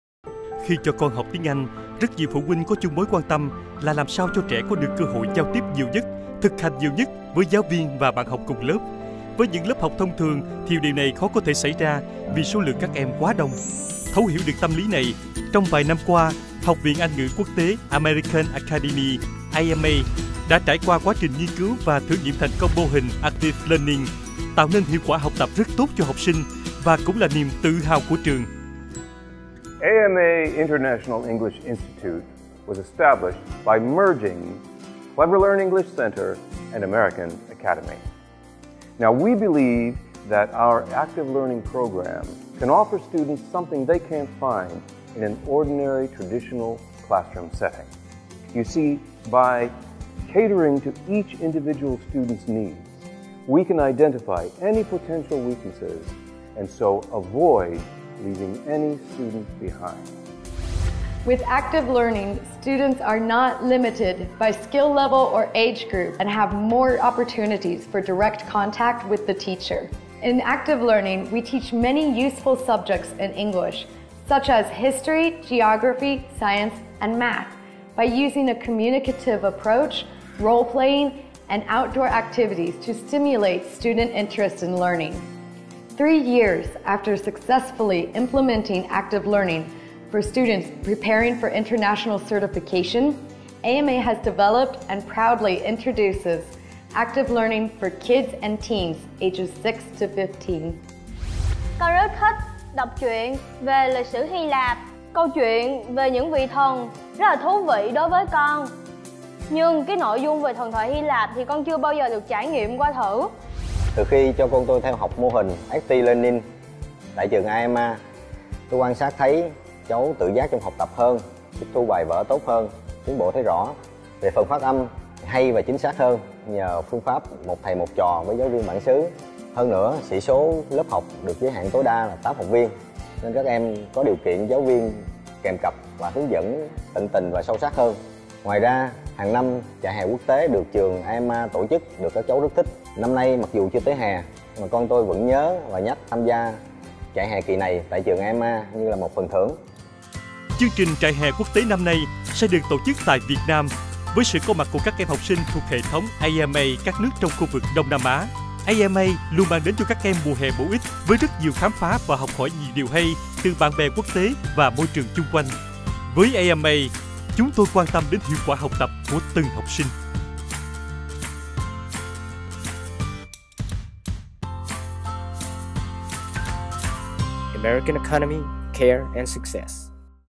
Phương pháp Học tiếng Anh chủ động Active Learning Kids Teens
Active Learning Kids Teens là chương trình học tiếng Anh của trung tâm Anh ngữ AMA dành cho các em học viên thiếu nhi, thiếu niên với phương pháp học tiếng Anh mới. Chương trình sẽ đem đến cho các học viên một môi trường dạy và học tiếng Anh năng động cùng nhiều hoạt động tìm hiểu, nghiên cứu khoa học - môi trường. Từ đó, các em sẽ tự nhiên yêu thích môn tiếng Anh và hình thành thói quen chủ động học tập, nghiên cứu để không ngừng nâng cao kỹ năng, kiến thức.